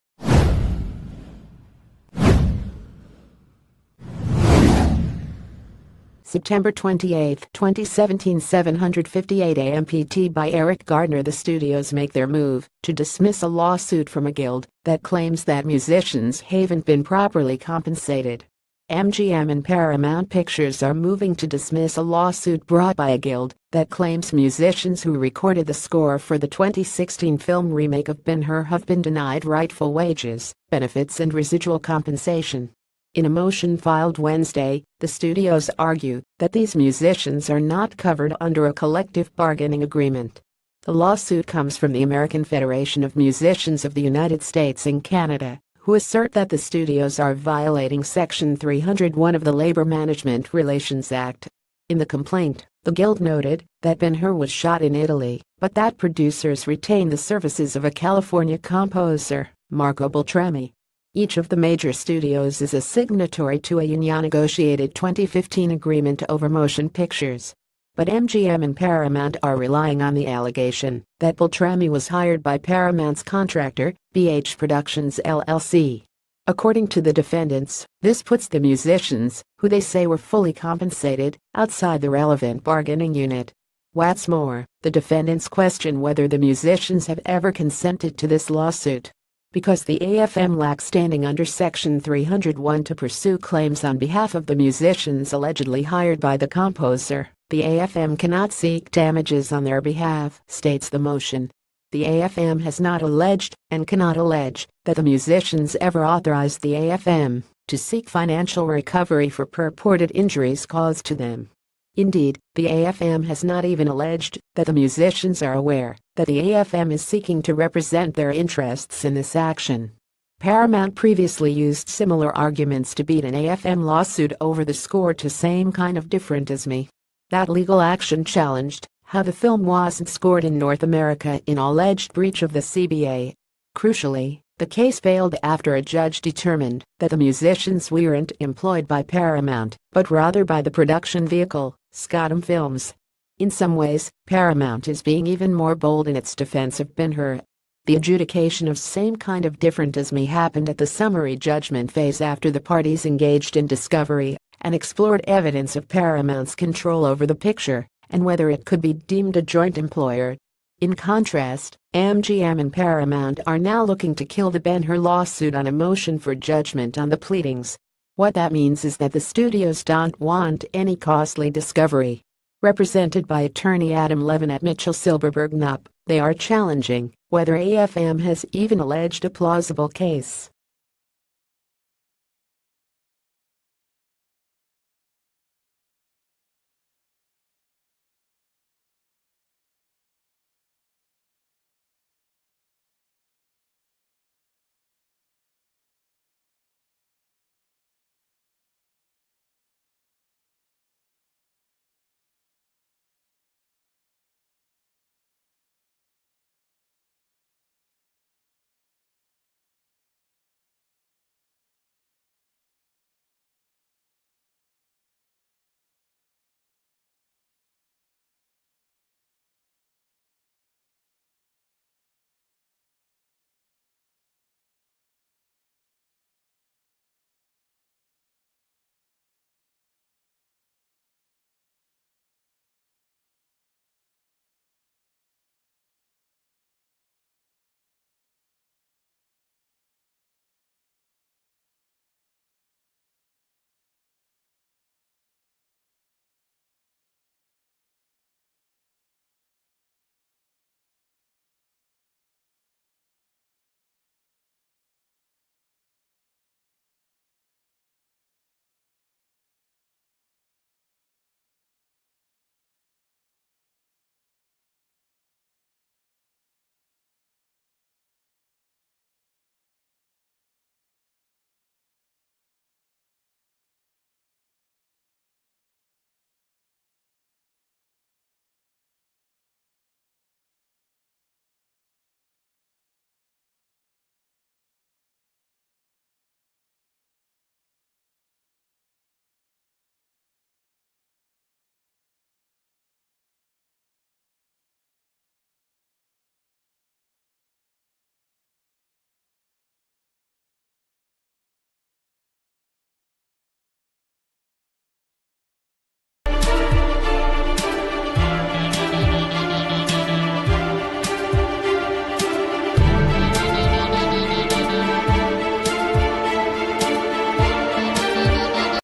Breaking News | Mgm, paramount say 'ben hur' musicians not covered by union agreement
Mgm, paramount say 'ben hur' musicians not covered by union agreement

September 28, 2017 758am PT by Eriq Gardner The studios make their move to dismiss a lawsuit from a ...